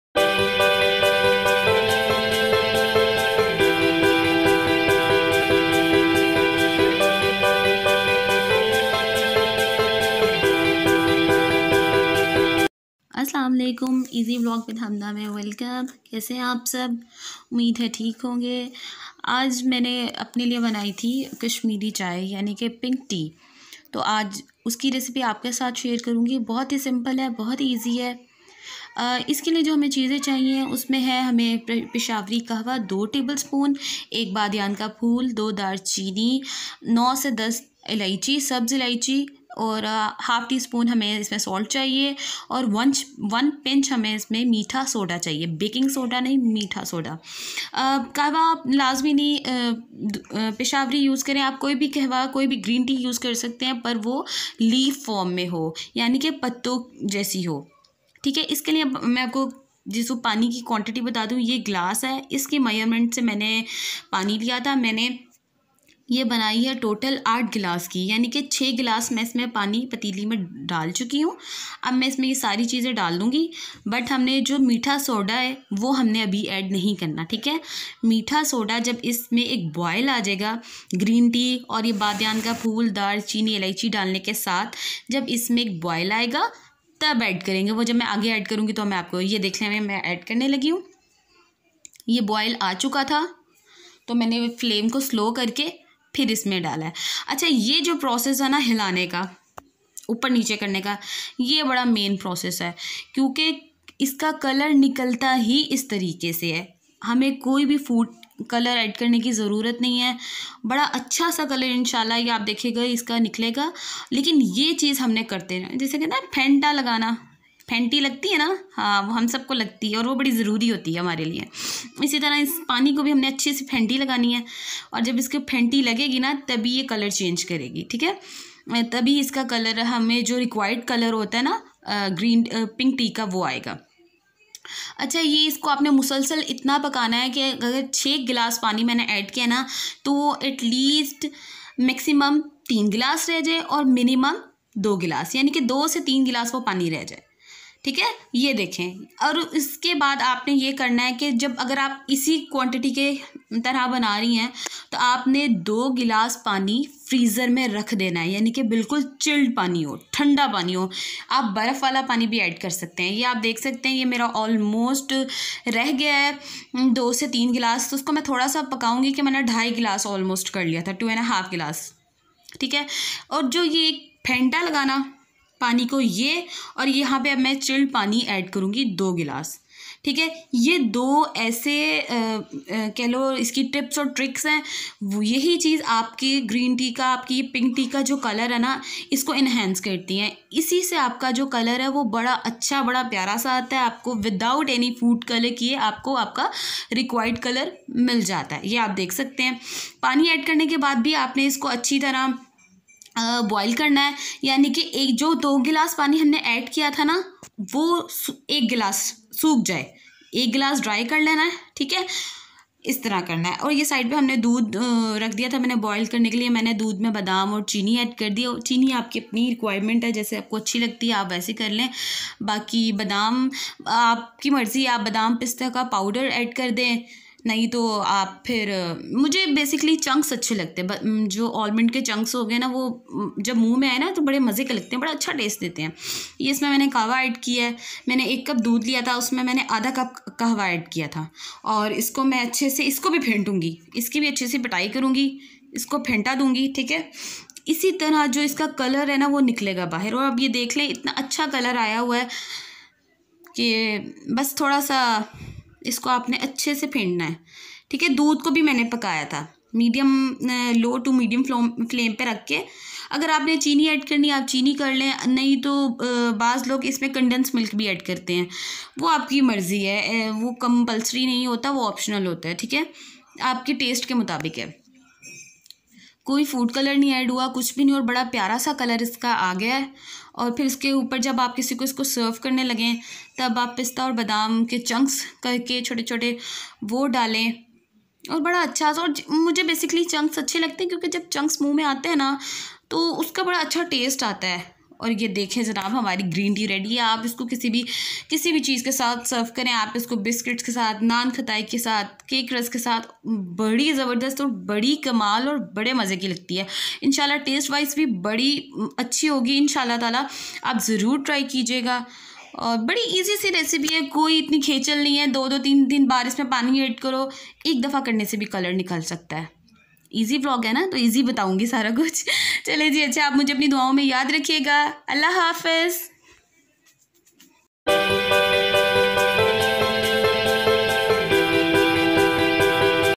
Kashmiri chai/Pink Tea
published on 12 December
video content
Kashmiri pink tea

Easy vlog with hamna
Aoa frnds

how are u? I hope u all r fine..

Kashmiri chai
pink tea

plz watch my full video Kashmiri pink tea..is is very easy Recipe of pink tea/kashmiri chai..Quick and v easy method to make pink tea. so. plzzzzzzzz frndz must try my recipe I hope u will enjoy my pink tea recipe and its taste too..INSHAH ALLAH..

ALLAH HAFIZ